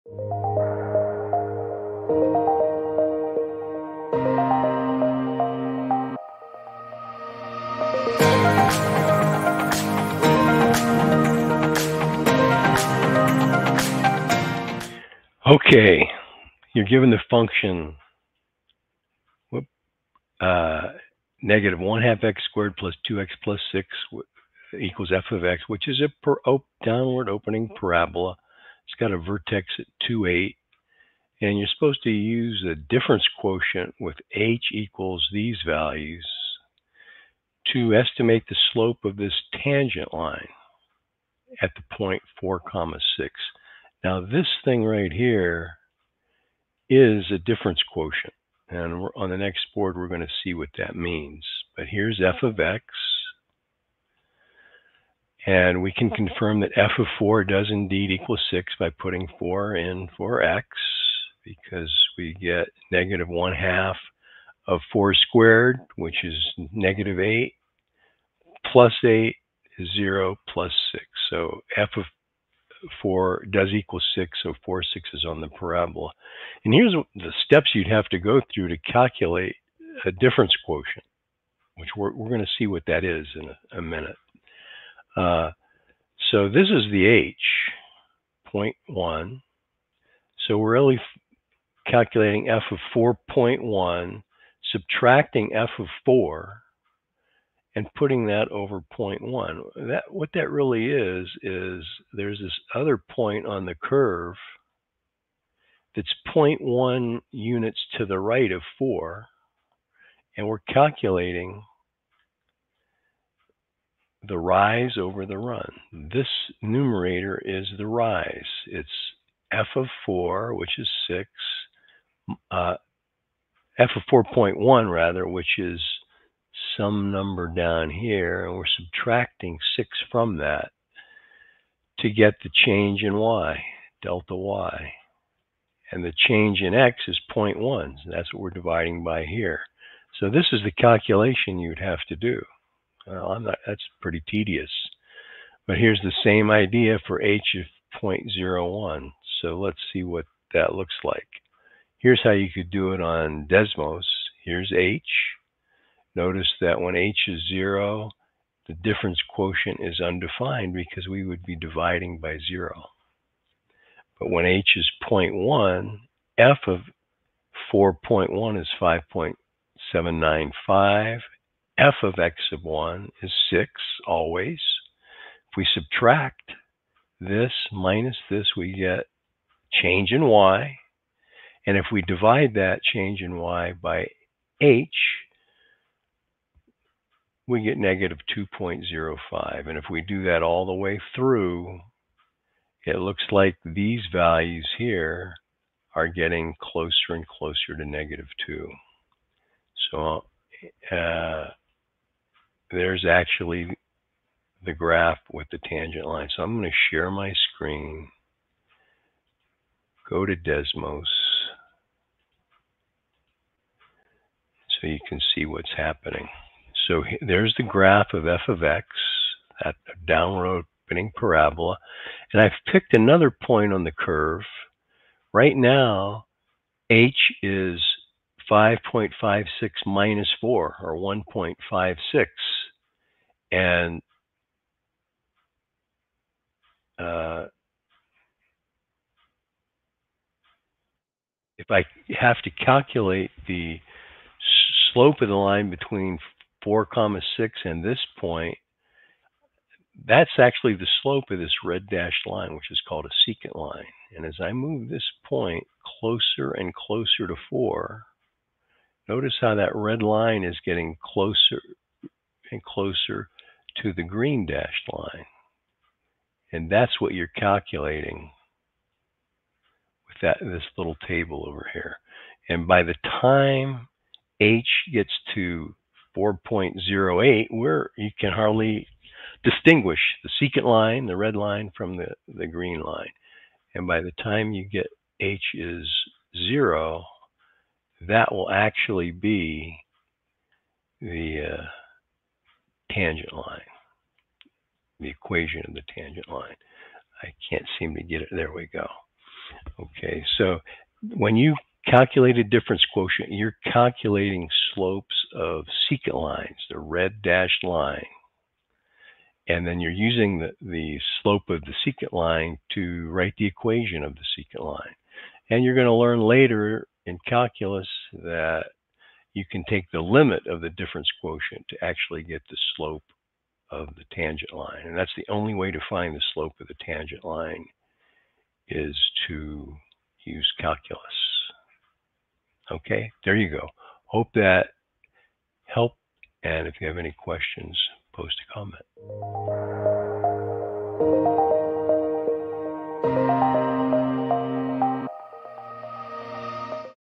Given a quadratic function calculate a difference quotient value for a given x value and h values.
Question: How do you do this?
Calculate the difference quotients for f(x)=6+2x−x^2/2 using h=0.1, 0.01, and 0.001. Use the results to approximate the slope of the tangent line to the graph of f(x) at the point (4,6). If necessary, round the difference quotients to no less than six decimal places and round your final answer to the nearest integer.

Answered By:

Doug C.